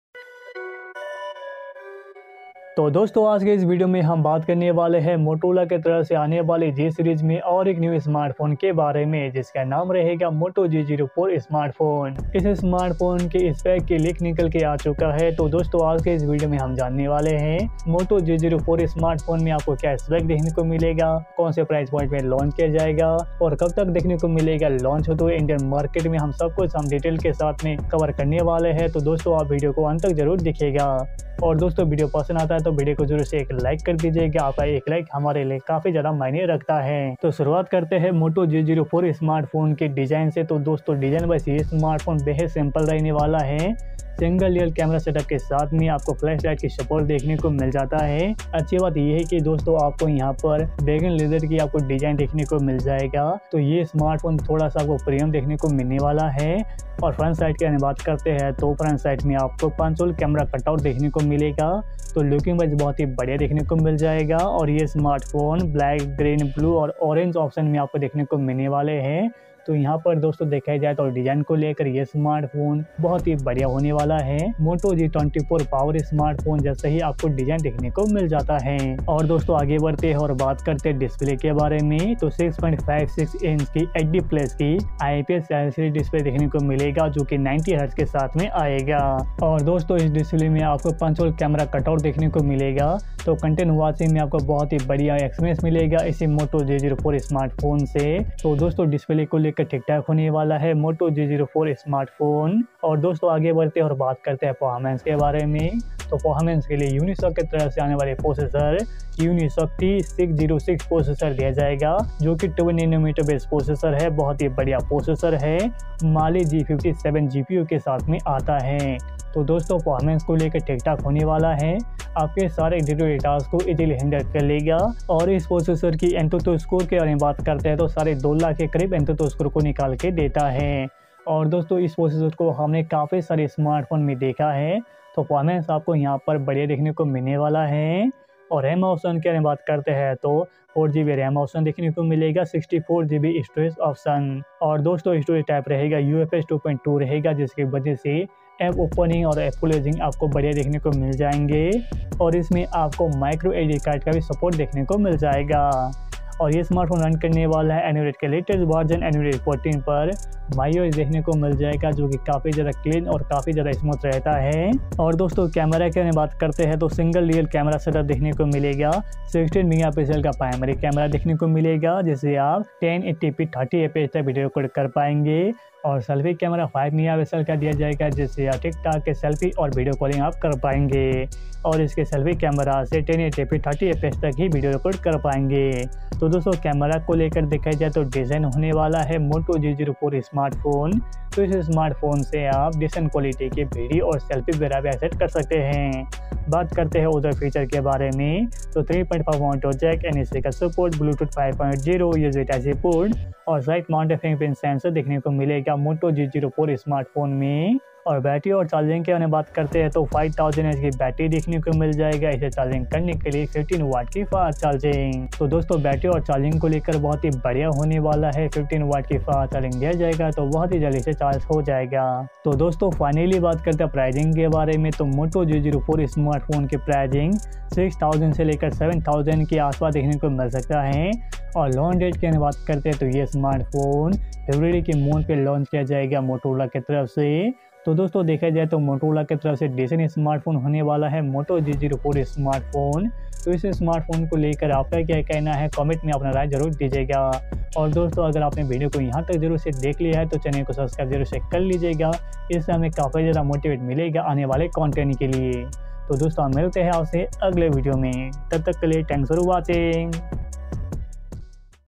Moto G04 - First Look | Specs | Price in india |Moto G04 Unboxing
The Moto G04 is a budget-friendly Android smartphone manufactured by Motorola. It features a 6.6-inch display with a resolution of 720 x 1600 pixels and a 90Hz refresh rate for smoother interactions. The phone is powered by a unisoc t606 processor and comes with 4GB of RAM, providing decent performance for everyday tasks.In terms of photography, the Moto g04 boasts single-camera setup on the back, including a 16MP main camera. For selfies, there is a 5MP front-facing camera.The device runs on a near-stock version of Android, offering a clean and user-friendly interface. It has a generous 5000mAh battery, ensuring extended usage between charges. Additionally, the Moto G04 supports fast charging for quick replenishment of the battery.Connectivity options include 4G LTE, Wi-Fi, Bluetooth, and a USB Type-C port. The phone typically comes with various storage options, and you can expand it further using a microSD card.